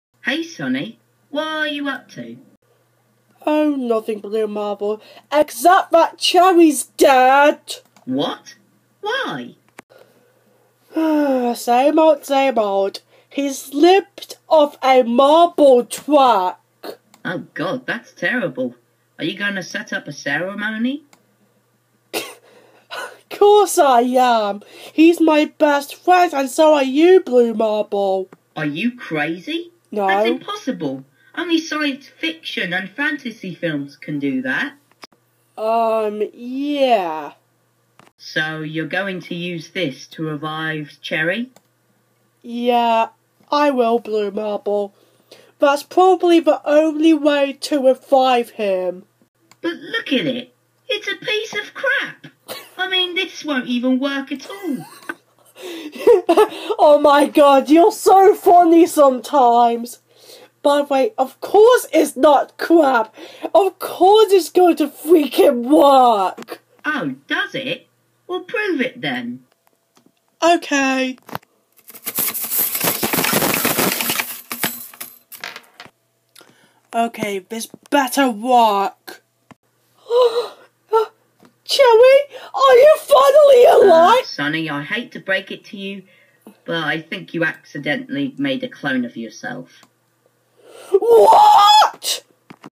SNEAK PEEK: New Marble Show (S4 E1) (SPOILERS)
JHR Rating: 7+
Contains mild language

Hey guys, HappyFace is back with the forth season of New Marble Show.
Here is a sneak peek of the first episode, coming February.

And very special thanks to Max Andrew for voicing Blue Marble.